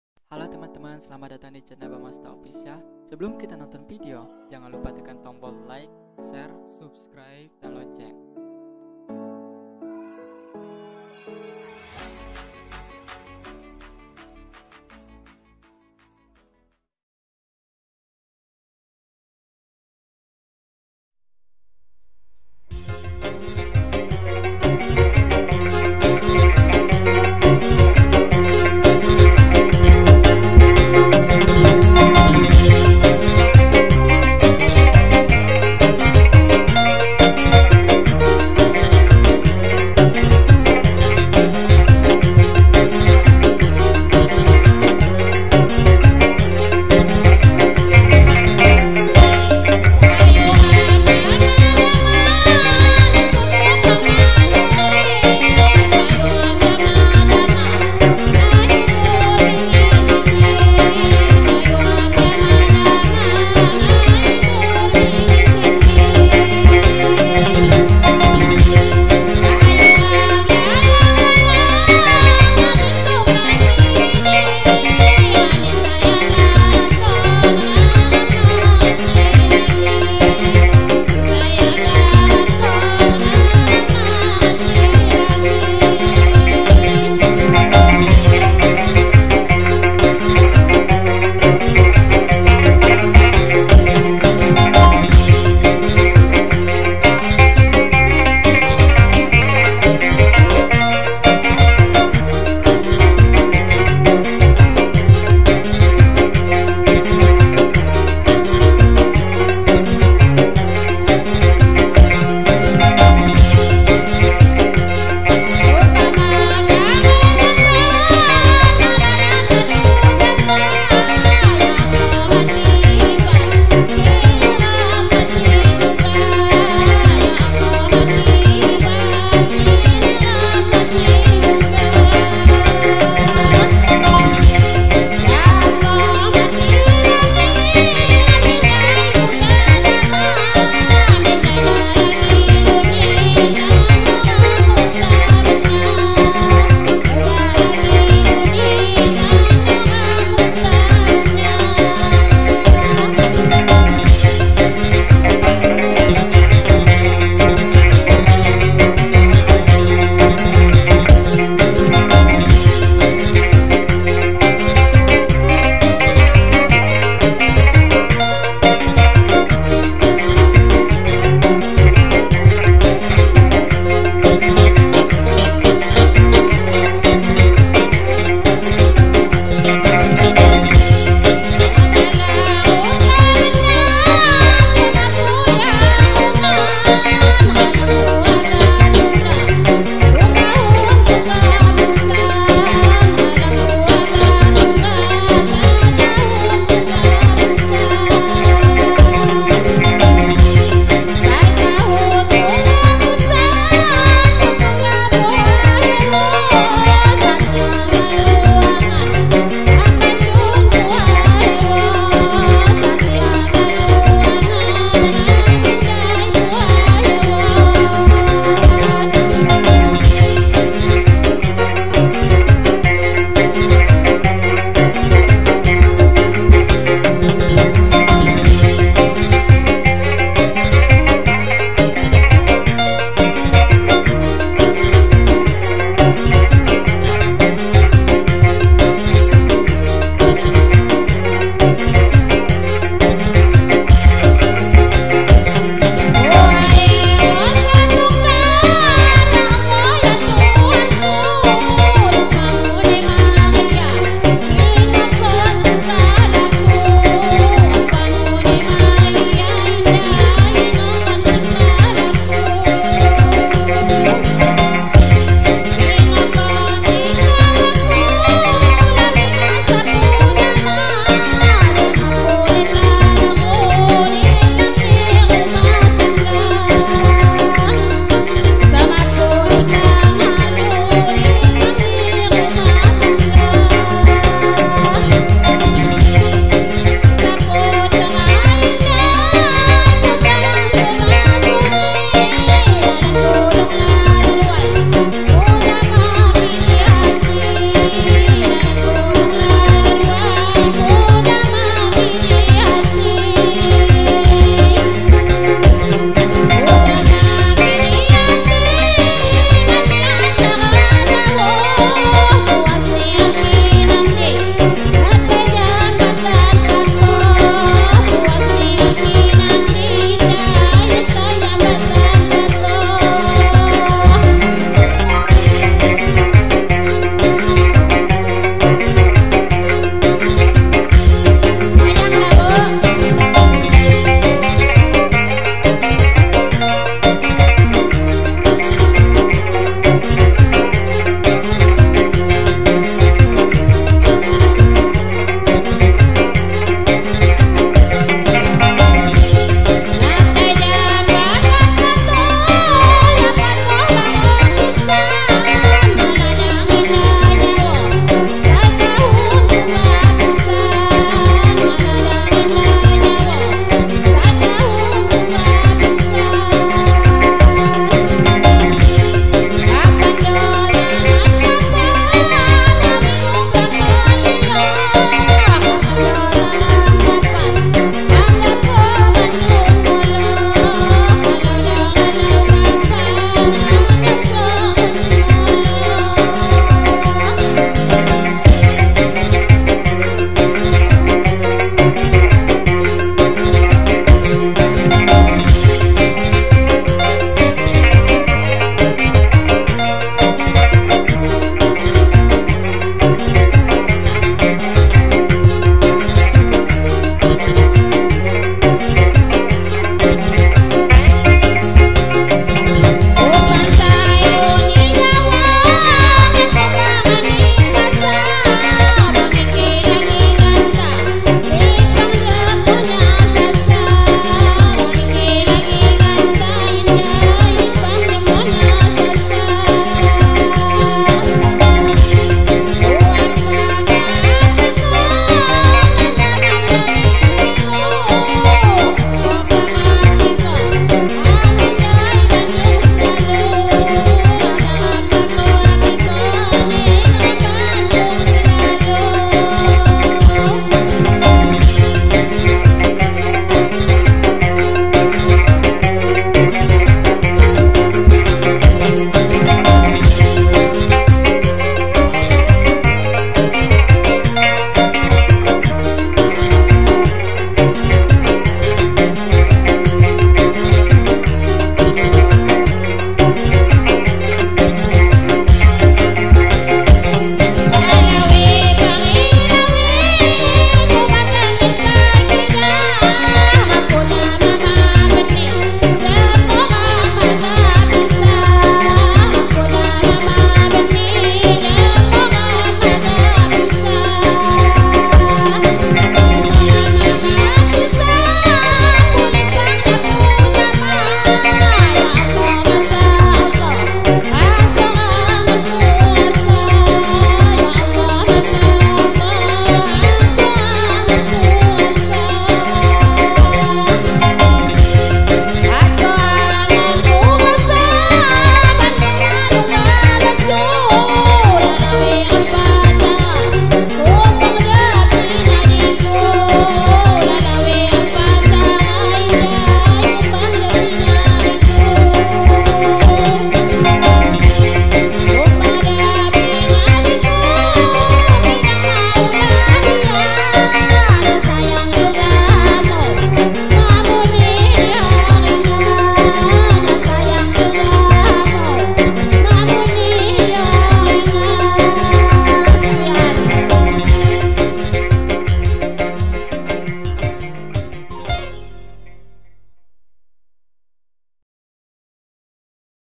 Lagu Tumbai Lampung || Ibu ibu Gambus Viul Orkes Adat Lampung | Enak didenger
Orkes New BAMASTA MUSIK ENTERTAINMENT
Selamat mendengarkan sajian lagu Daerah, Lagu dangdut pop koplo dari New Bamasta

 0⃣8⃣5⃣3⃣8⃣1⃣0⃣5⃣4⃣3⃣9⃣3⃣
 0⃣8⃣2⃣2⃣8⃣0⃣1⃣8⃣3⃣1⃣8⃣4⃣

By Angga BAMASTA